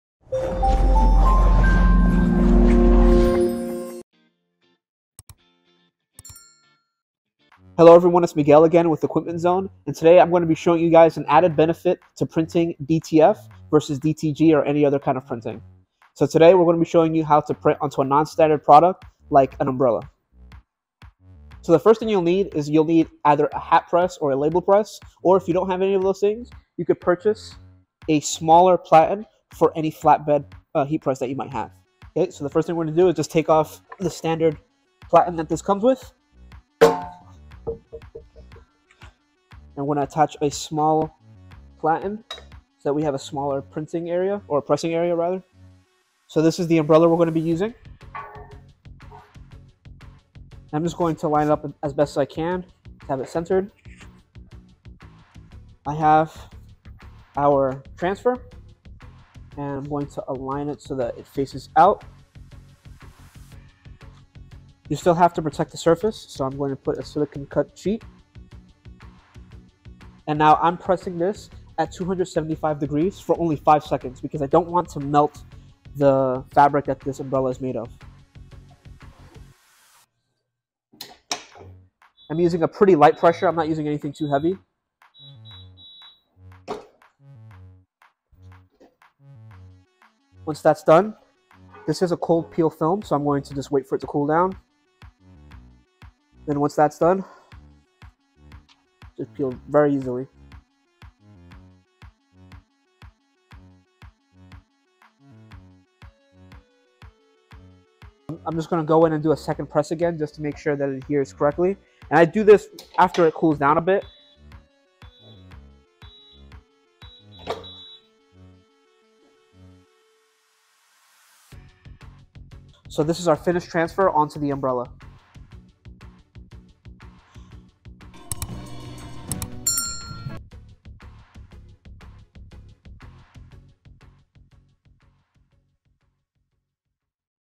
YES You Can Print Umbrellas With DTF Transfers! ☔
The techs at Equipment Zone are always looking to show you the variety of products you can decorate using direct to film transfers. Here is one of the hardest items to print successfully - nylon umbrellas. See how easy it is to get great looking results using DTF transfers printed on either our EZ-JET PRO DTF Printers or our Epson F2100 or F3070 DTG/DTF Printers.